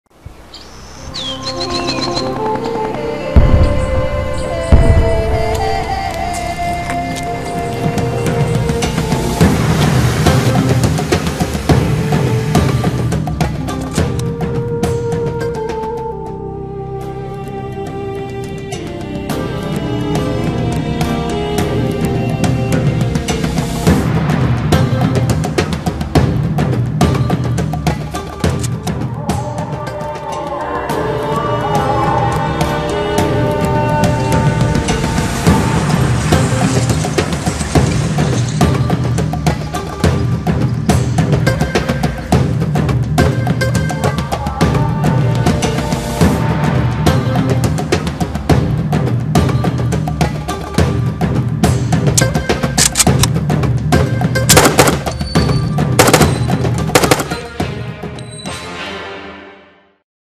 Beryl wz. 96
Karabin Beryl z Fabryki Broni "Łucznik" Radom. Dobre, bo polskie